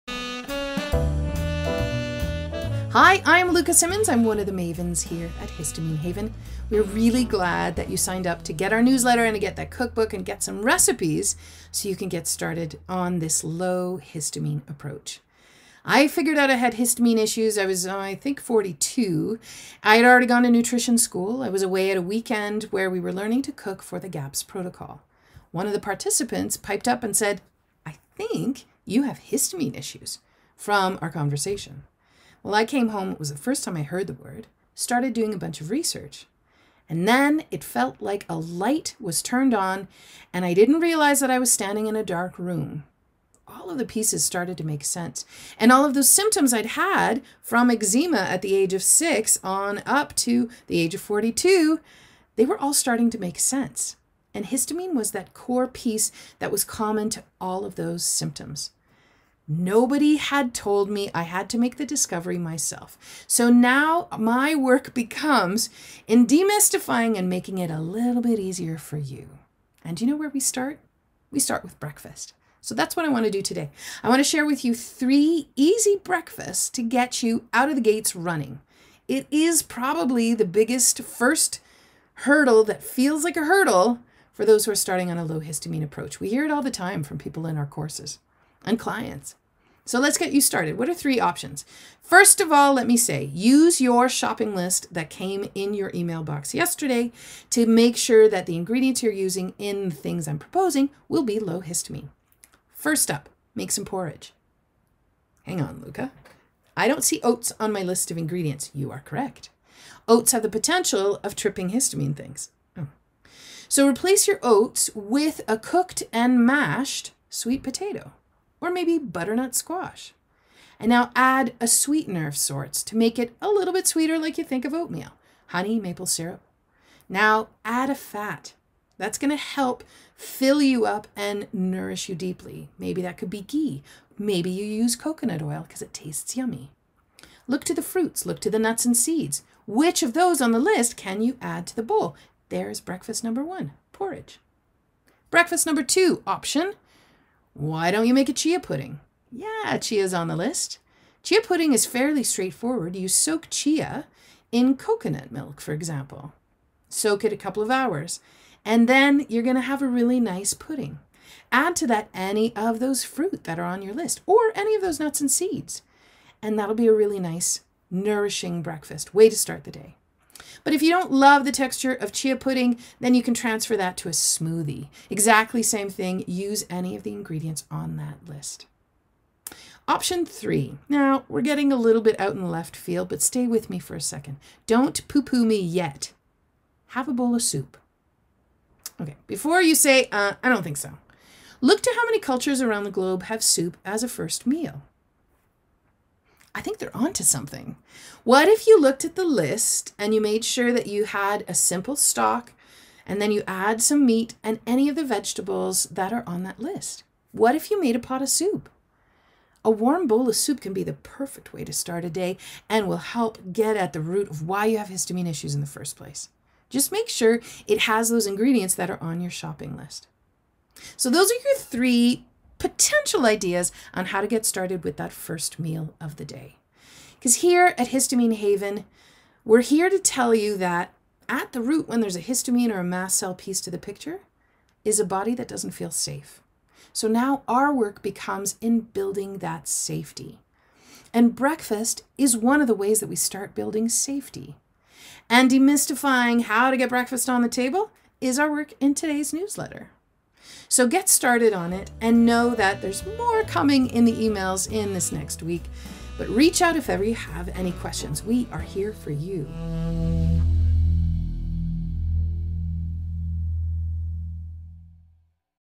3 low histamine breakfast ideas
And a bit about Luka, one of the mavens at Histamine Haven. Missed finding the Shopping List?

@histaminehaven on the socials